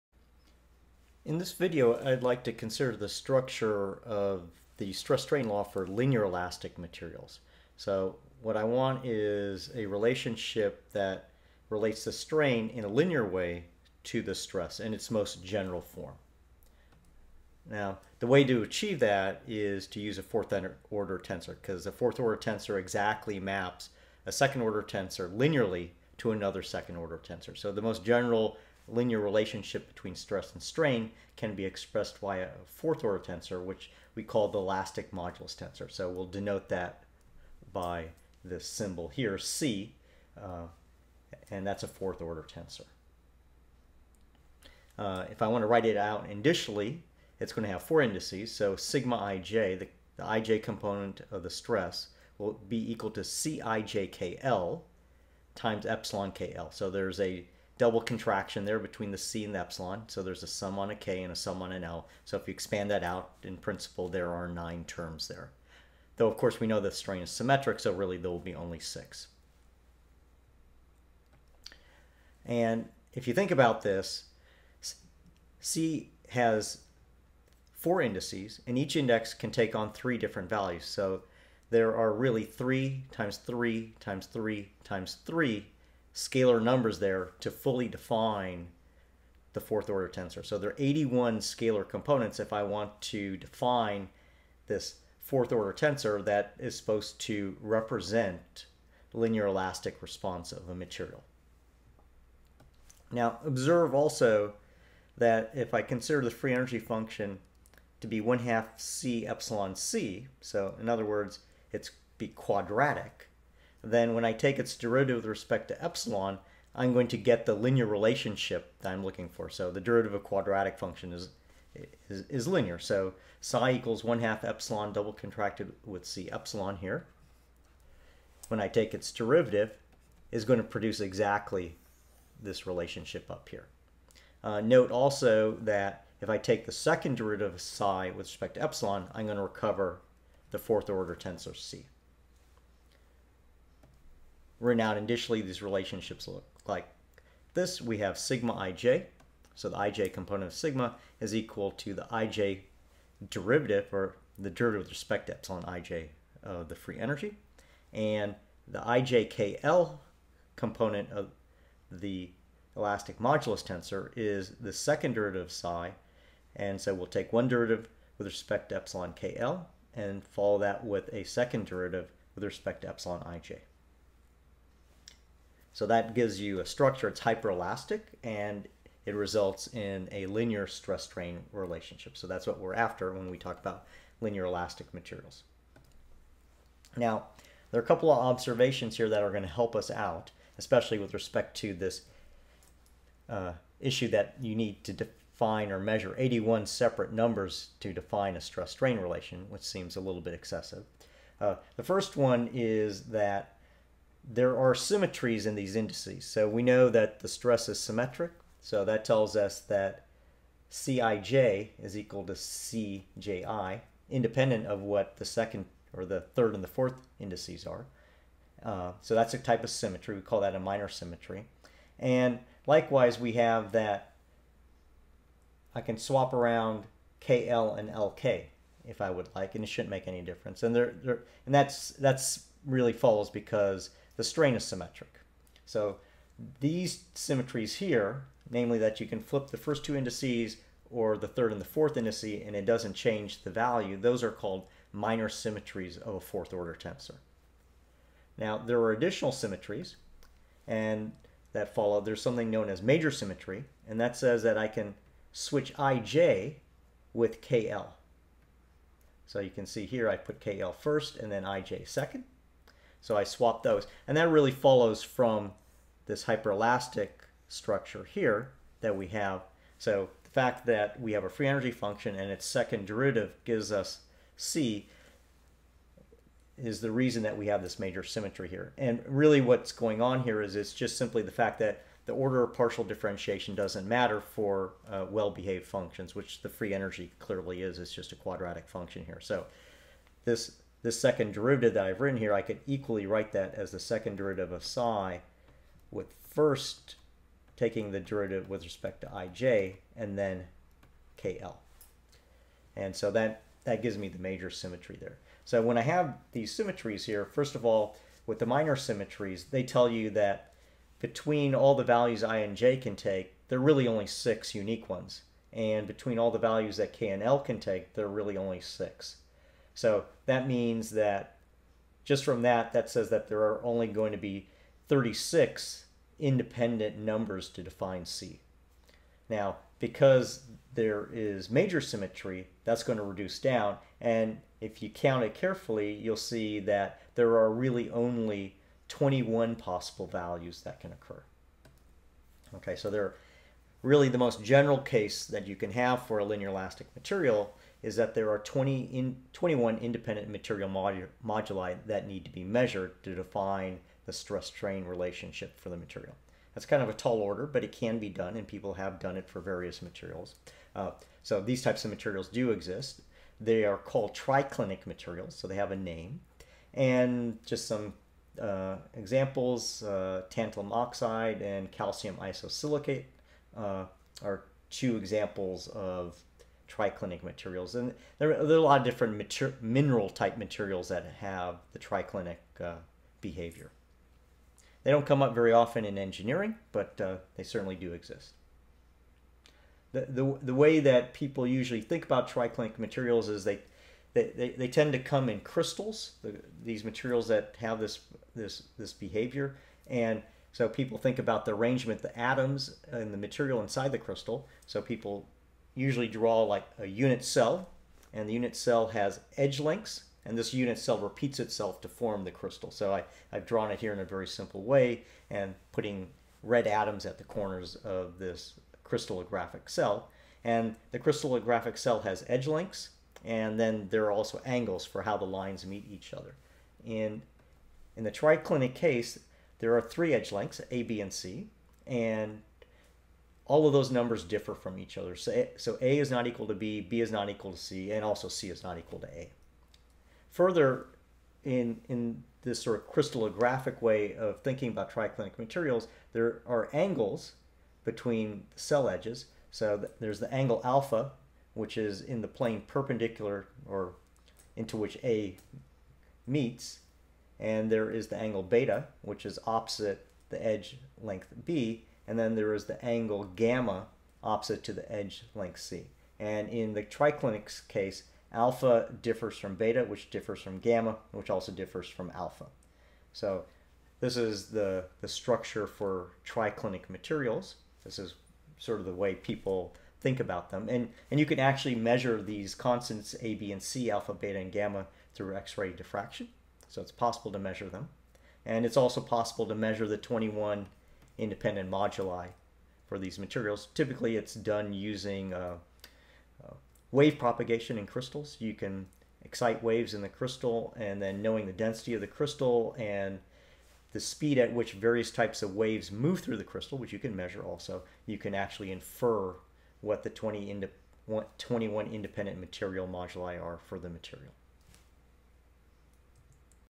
25. Linear elastic constitutive law (stress-strain relation)
The general form of stress-strain relation for the most general linear elastic material class, triclinic materials.